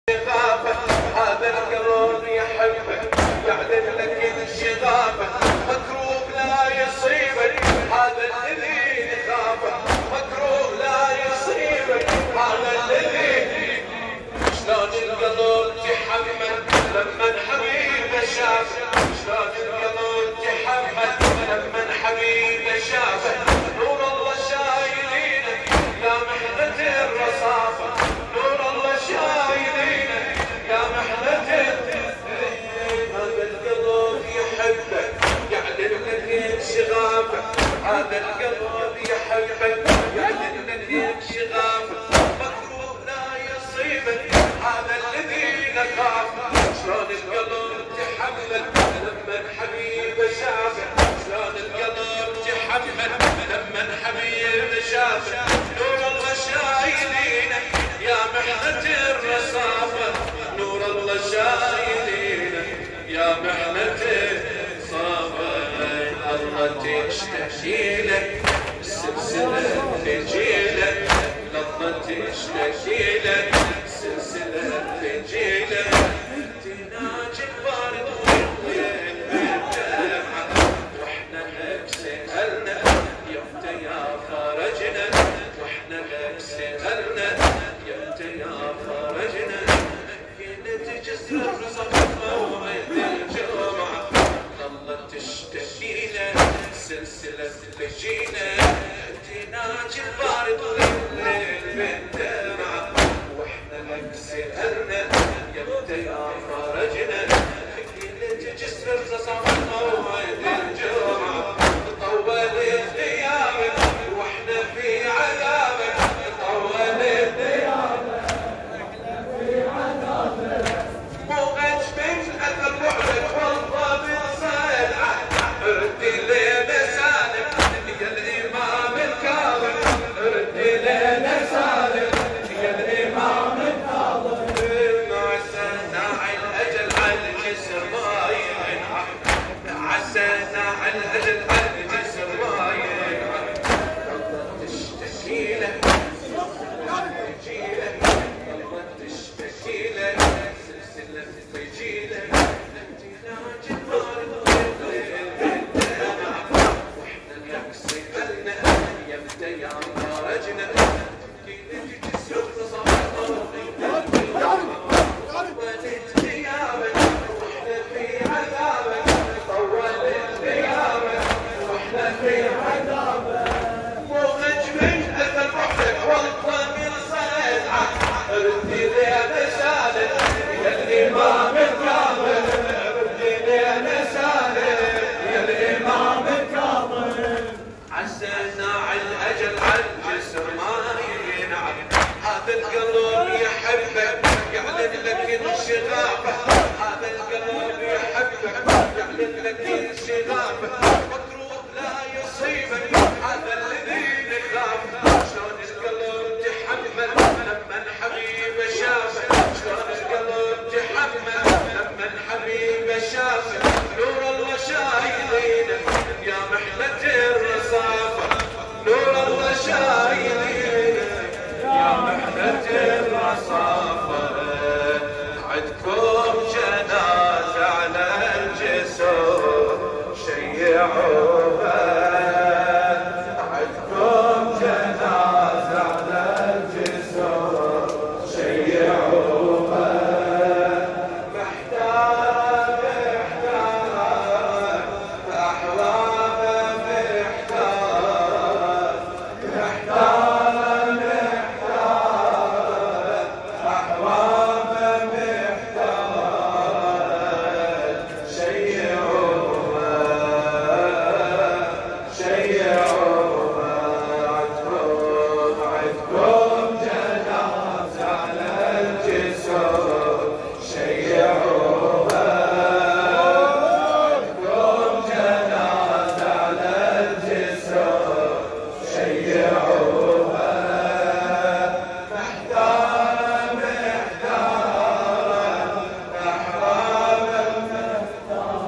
علي حماد | ليلة وفاة الإمام الكاظم عليه السلام | مأتم بن سلوم، 25 رجب 1440 هـ
علي_حماد ليلة وفاة الإمام الكاظم عليه السلام | مأتم بن سلوم ليلة 25 رجب 1440 هـ